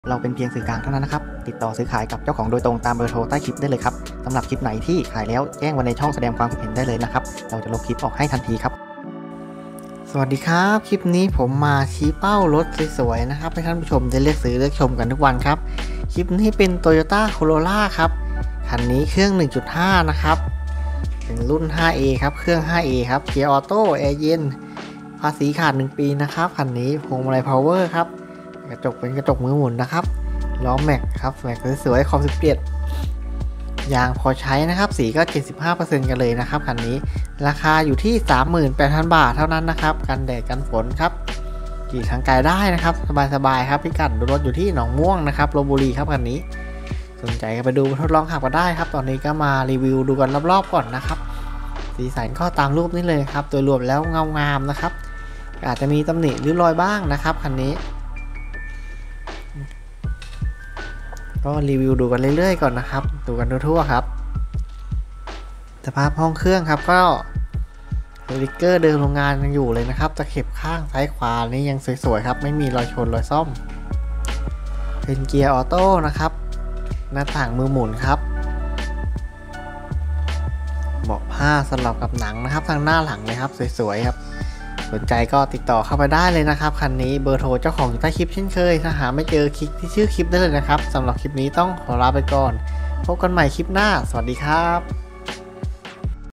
toyota corolla เกียร์ออโต้ราคาถูก
1993 Toyota Corolla

฿38,000

TOYOTA COROLLA เครื่อง1.5 (5A) auto แอร์เย็น
ภาษีขาด1ปี เล่มโอน ก.มือหมุน พ.พาวเวอร์
เลี้ยวไม่ดัง แม็ก17"ยางพอใช้ สี75%
ราคา 38,000บาท 081/2997167 ดูรถ หนองม่วง ลพบุรี

ติดตามรับชมช่องใหม่ของเราคลิกที่ลิงค์ได้เลยครับ 👇👉
🌳หรือแอดไลน์คลิกที่ลิงค์เลยครับ 👉 สำหรับติดต่อโฆษณาเท่านั้น 👉เบอร์เจ้าของสินค้าอยู่บรรทัดด้านบน